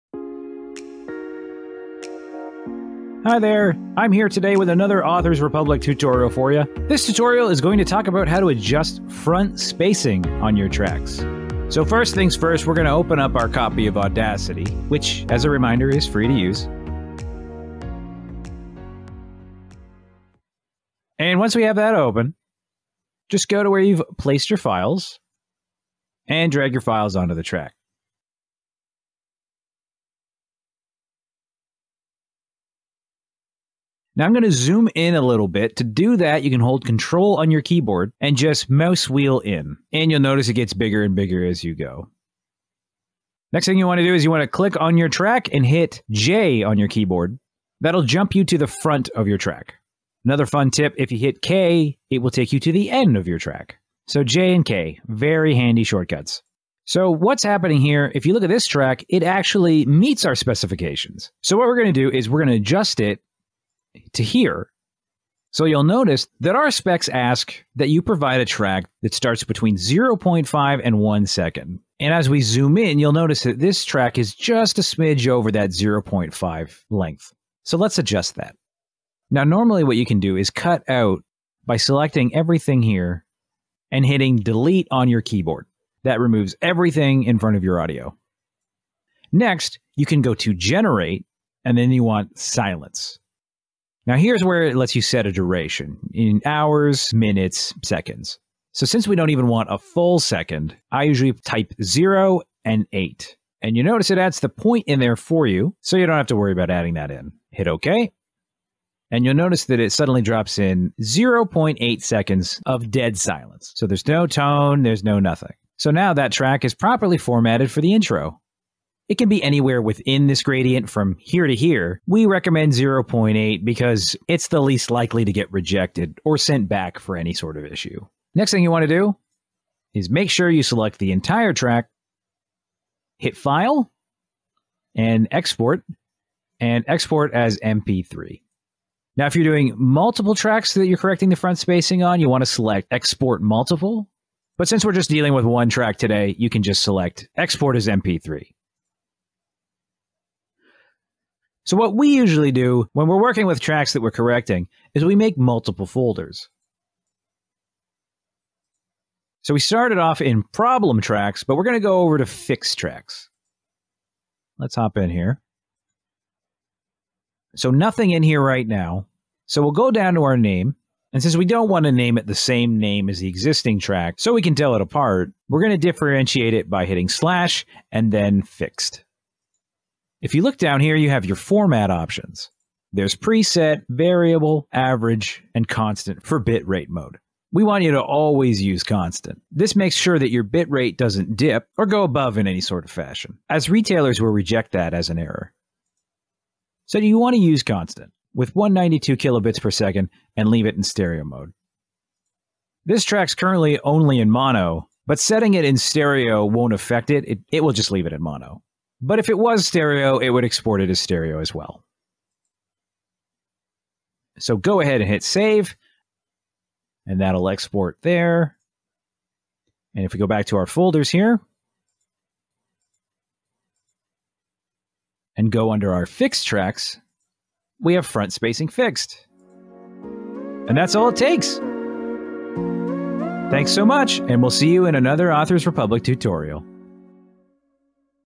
Front Spacing | How To Meet Audiobook Requirements | Audacity Tutorial 2.3.2
Struggling to meet audio requirements? Audiobooks must meet specific audio requirements to be submitted to retail, library and music platforms.

Each track must contain 0.5 to 1 second of silence at the beginning and 1 to 5 seconds of silence at the end. Follow this tutorial to set appropriate spacing for your audiobook tracks.